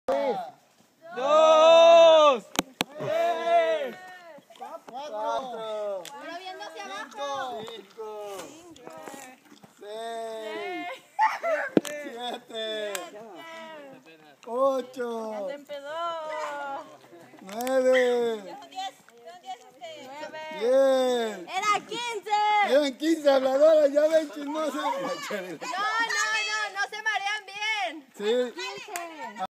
AEC Camping 2012
Chabelita y Chori jugando los borrachitos